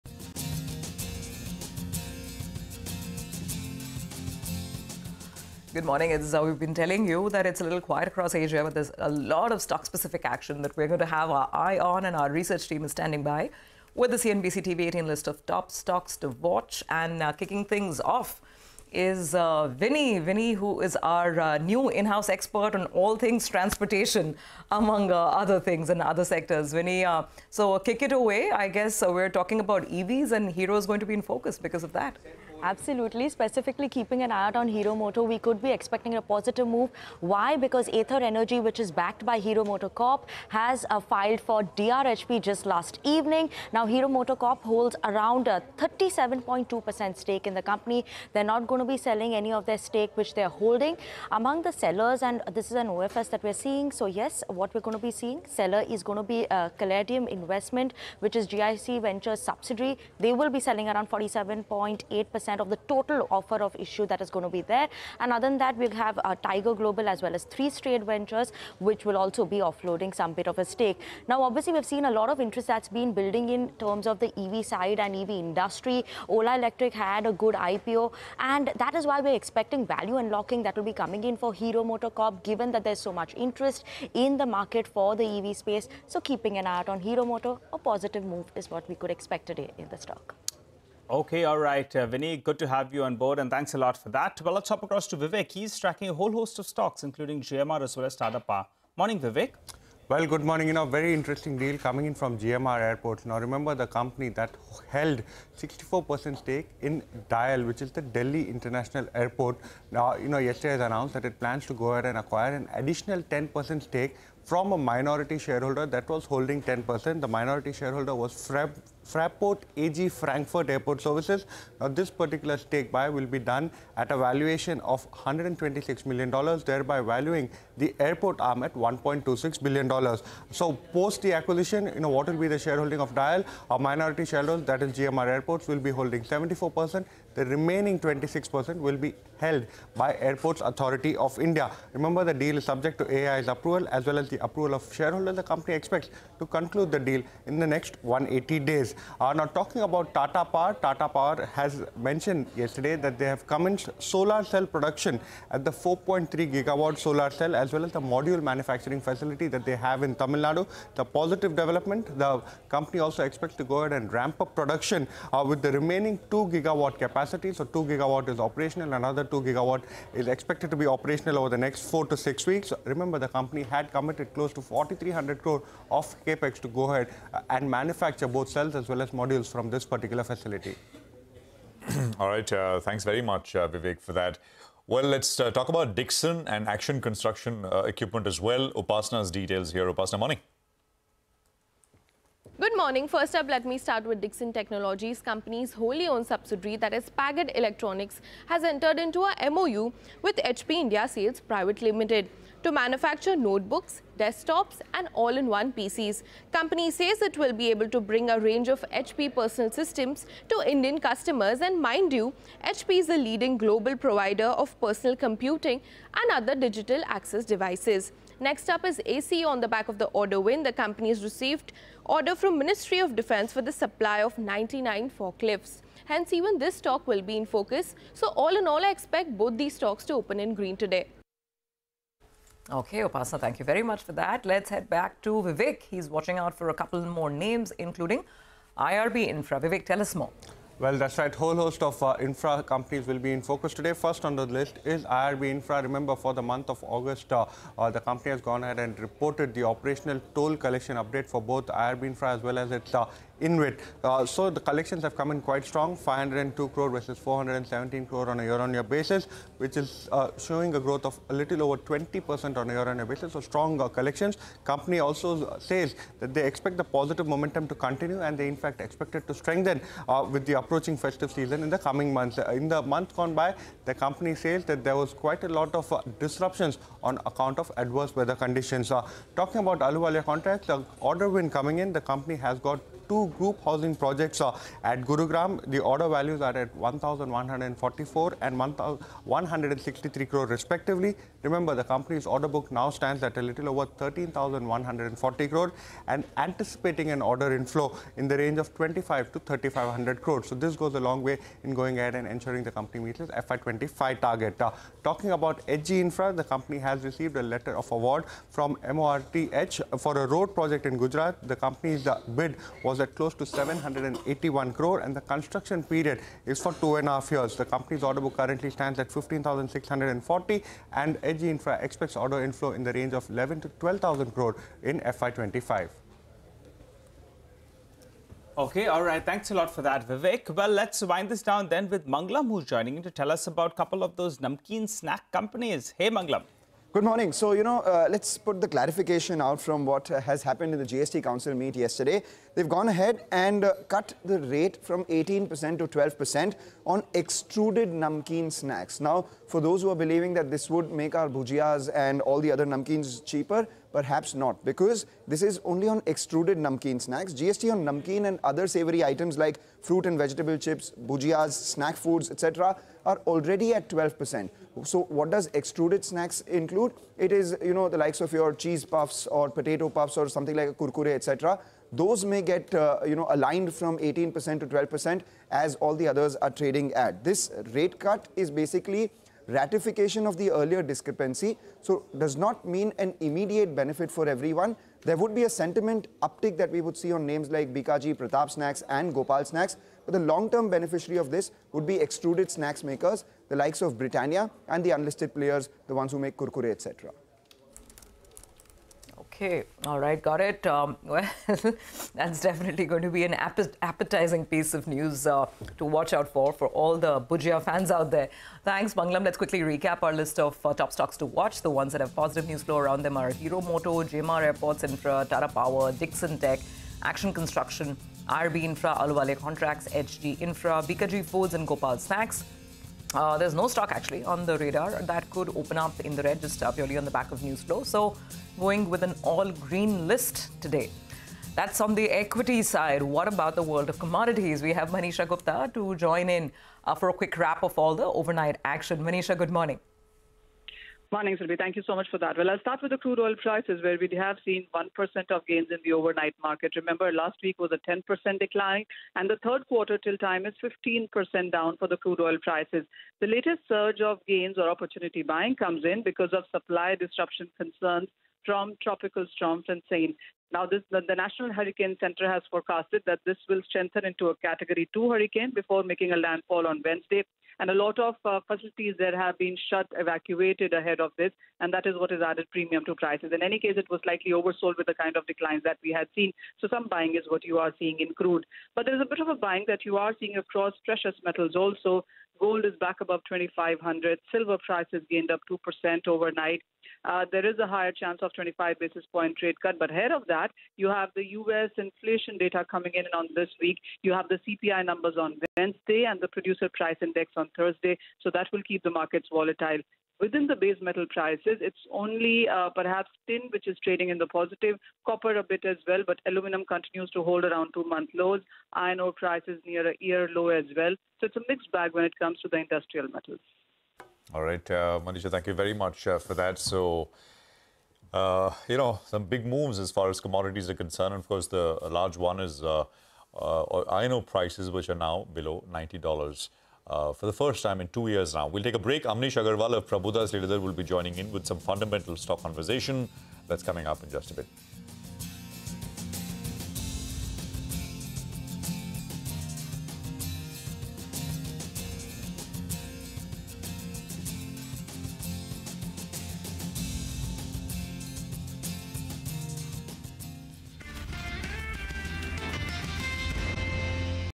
Key Stocks In Focus: Hero MotoCorp, GMR Airports, Tata Power, Dixon Technologies, ACE, IRB Infra
Here are the top 10 stocks you need to keep on your radar in today's trading session. Take a look.

n18oc_business